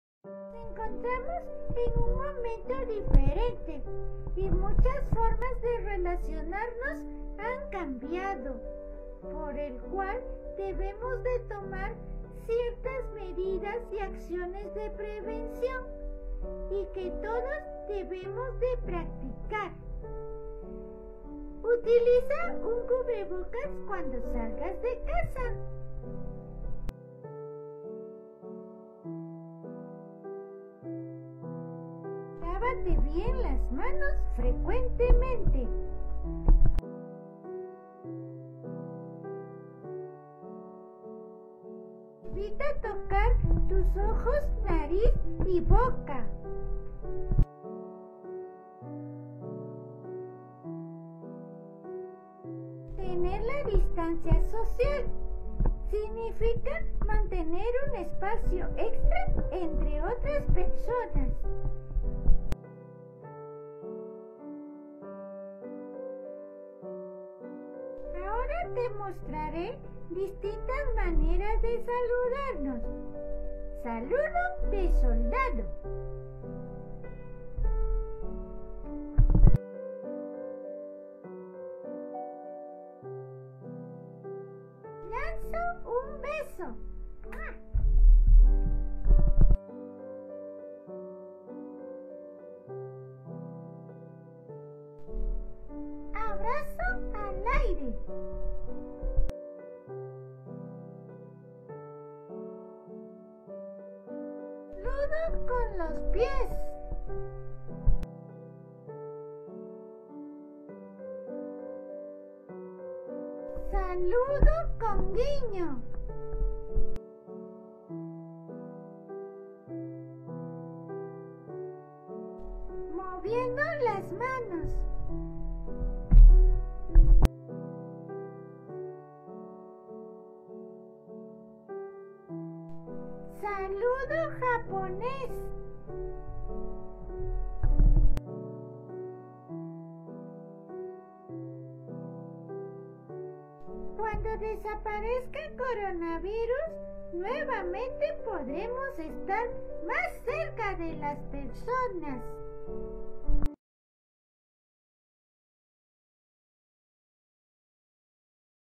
Distintas formas para saludarnos
Valito nos enseña las distintas formas de saludarnos sin tocarnos.